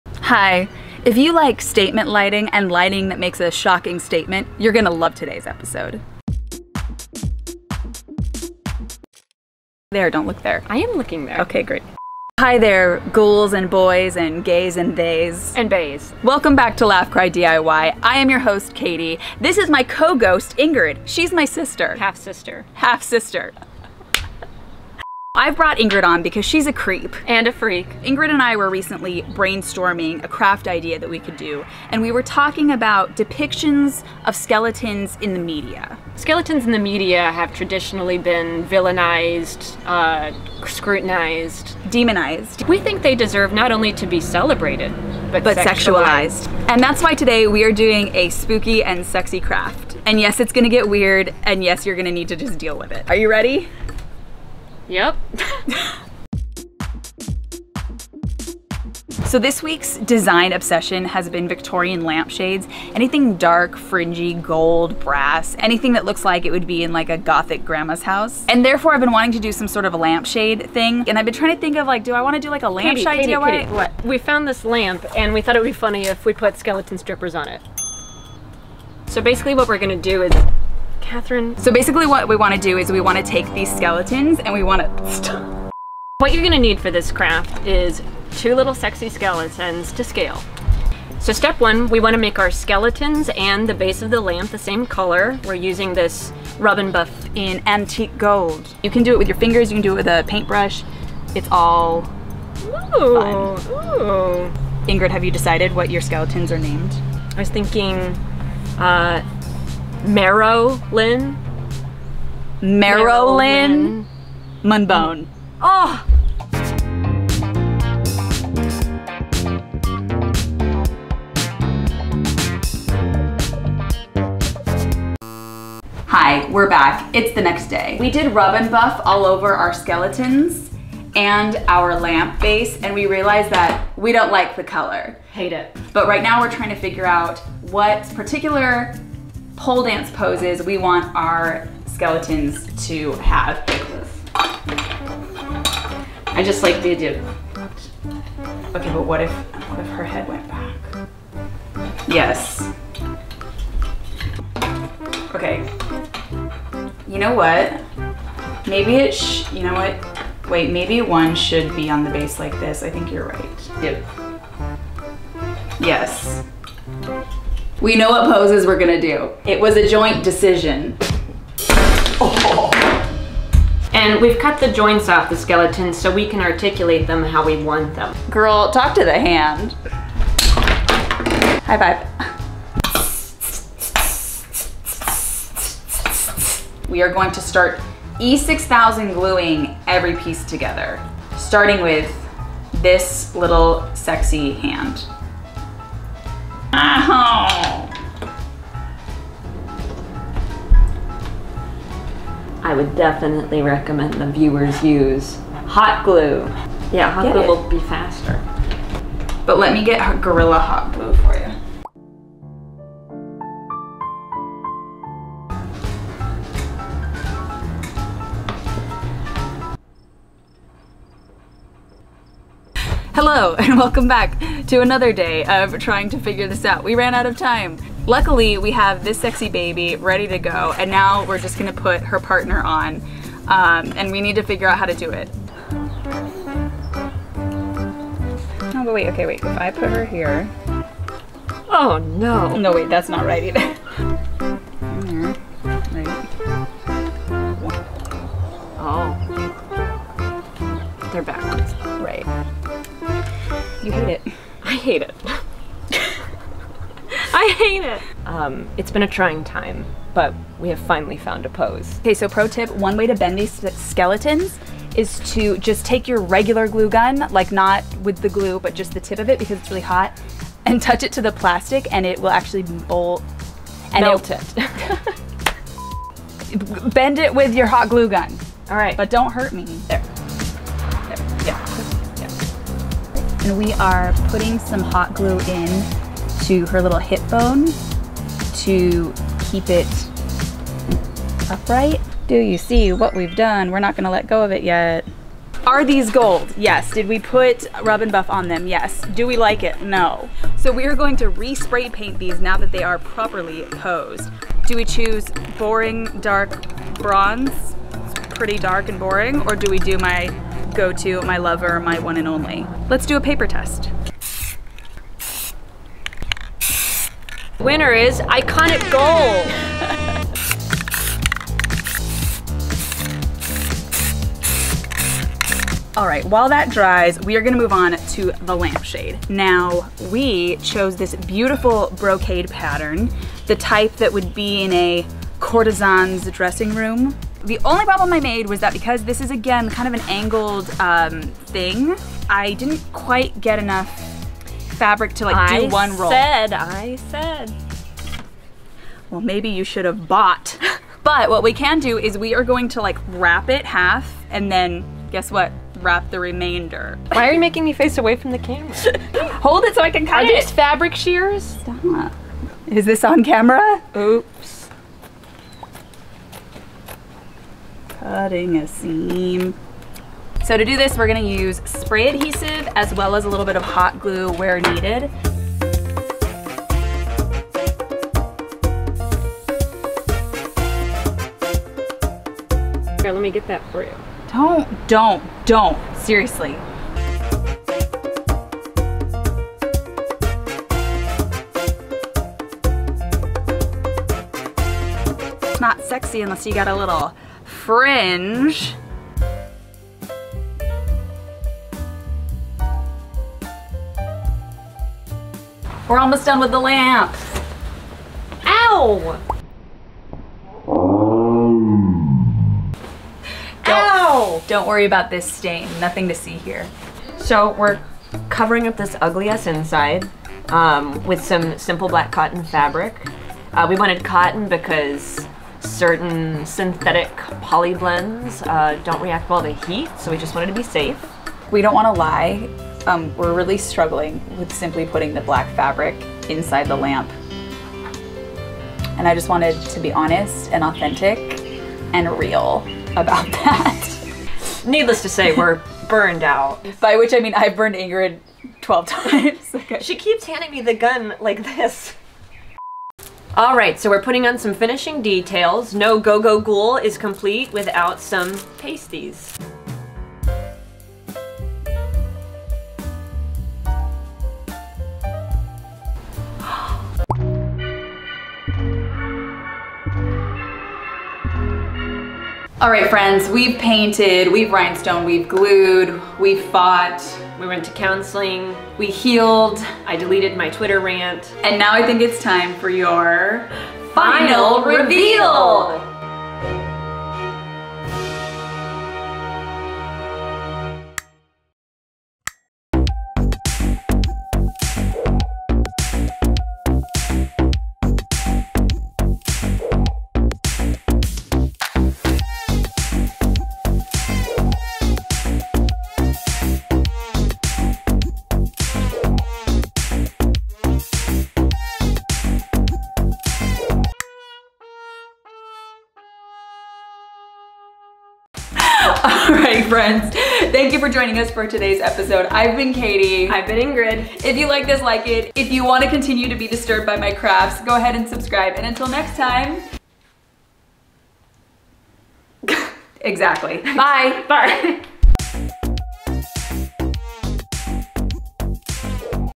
Making a Whimsigoth Sexy Pole-Dancing Skeleton Lamp | Fun Halloween Craft
☕️ LIKE MY CONTENT?

This week we’re celebrating Halloween by making it REALLY weird with this fun and sexy craft. Are there glamorous fabrics? Duh. Are there jewels? Yep. Are there skeletons pole dancing? Obviously. Watch my sister and I tackle this glamorous lamp with cheap skeletons, a clearance lamp, and a very weird and disturbing creative vision.

SUPPLIES
Plastic Skeletons
Spray paint
Lamp
Fabric
Hot glue gun
Fringe trim

FILMING INFO

SOUND
Wall light switch, on and off (StevenBrown)

Note: I am not a professional. All projects seen on this channel must be completed at your own risk. I do not take responsibility for any harm, injury, or subsequent drama that may occur.